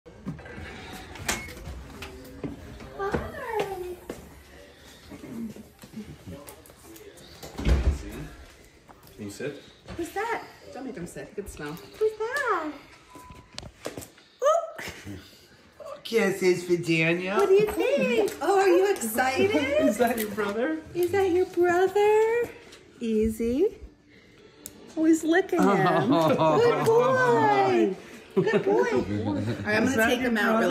Adorable Dog Meets Family's New Baby!
Yeah, this definitely made us cry happy tears! SO MANY HAPPY TEARS! 🐾🐶💕 (Via - Angela C.)